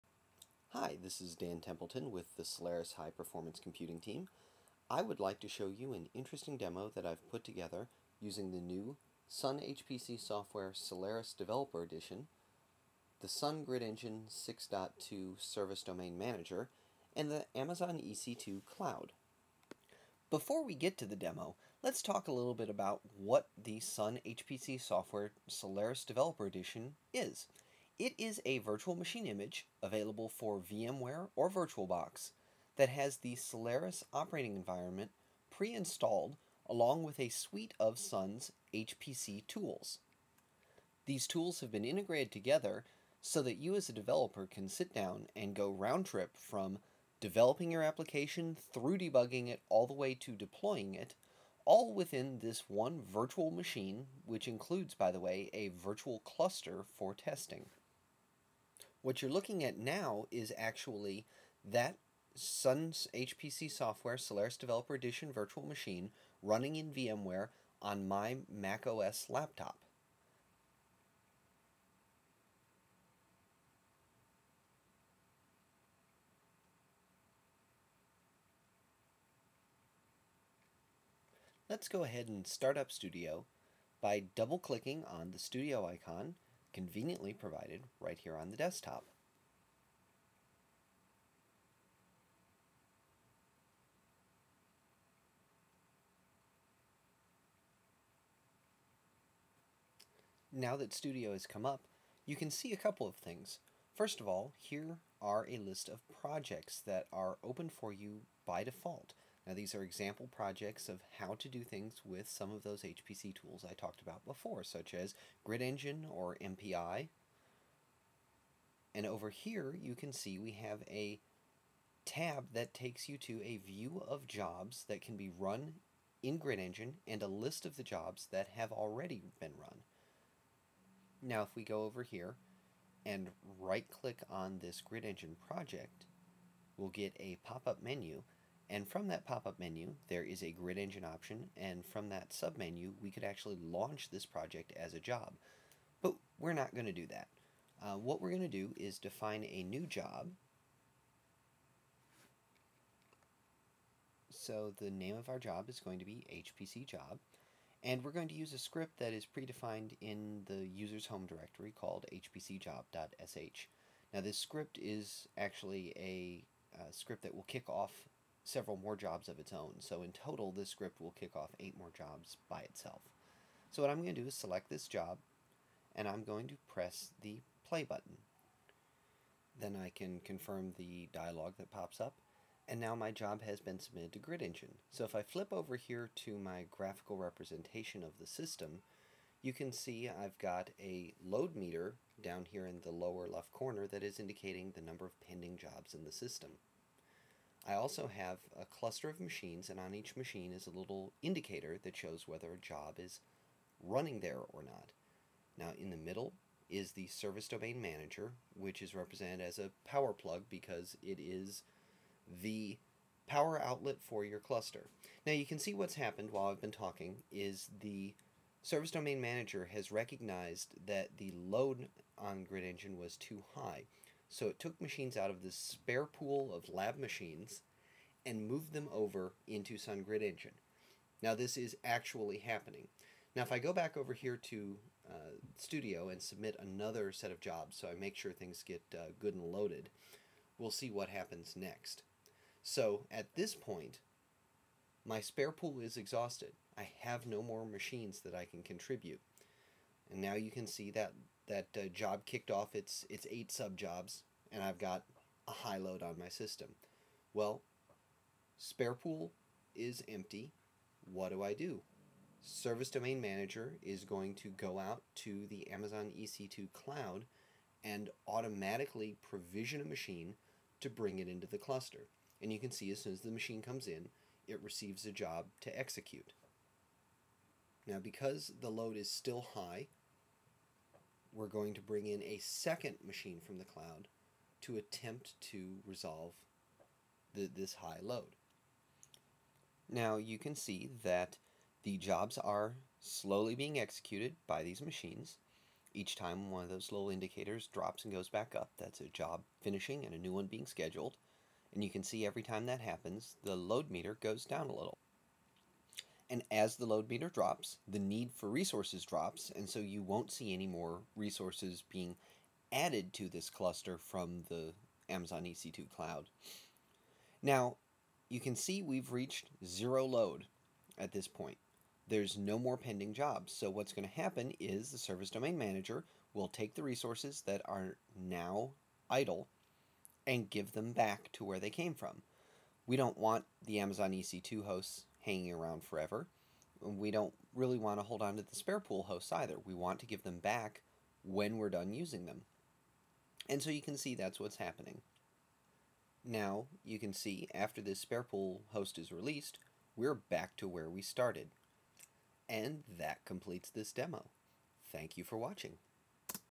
Sun HPC Software, Solaris Developer Edition & the Cloud
This is a demo of using Service Domain Manager to automatically borrow resources for the Sun HPC Software, Solaris Developer Edition from a local spare pool and the Amazon EC2 cloud.